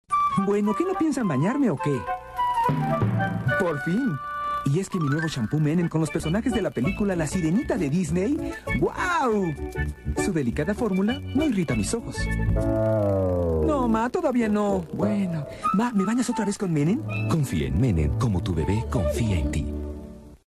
Anuncio shampoo baby magic con promocion de la sirenita. 1998
yo llegue a usar este shampoo solo porque me encantaba la sirenita.

Todos los derechos pertenecen a mennen y a quien subio originalmente el video.

link de mi blog